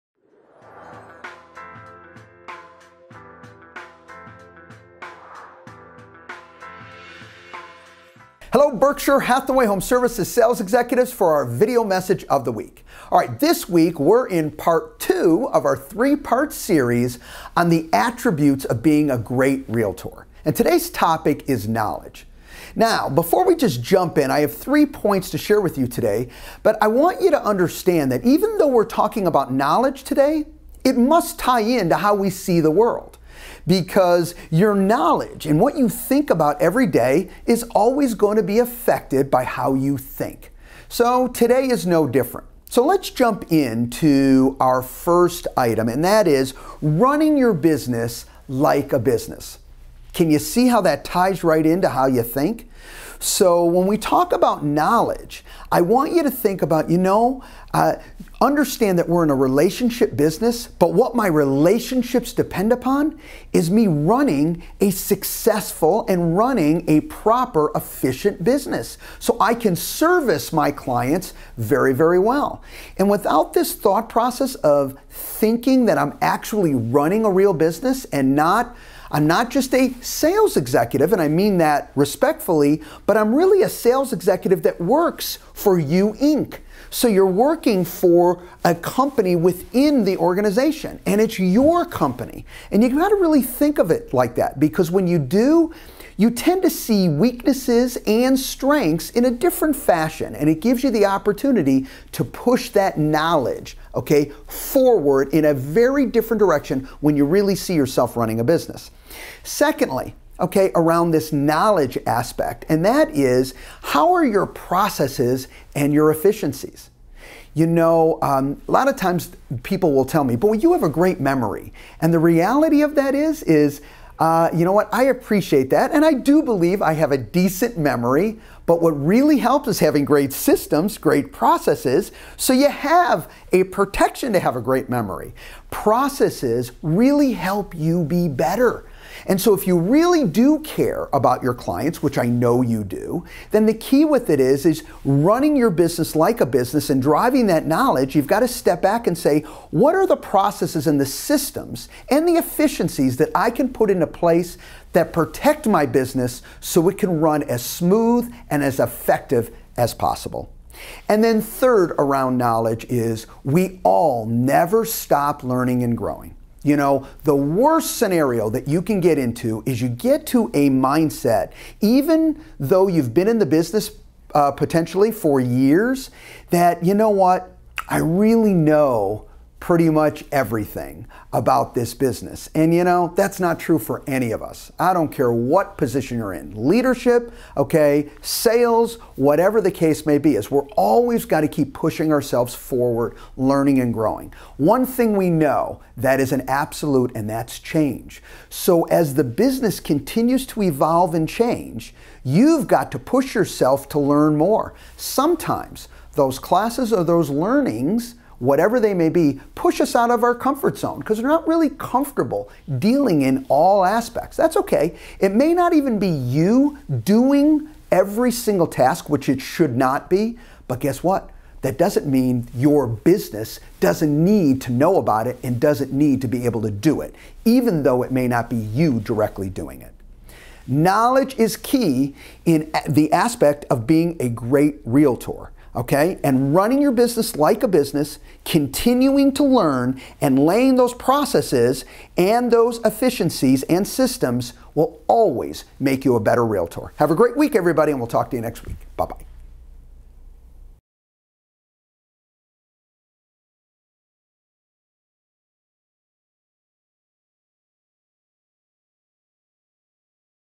Real Estate Training - Attributes Of A Great REALTOR Part 2 of 3
Knowledge

•Running your business like a business

•Processes / Efficiency

•Always Growing & Learning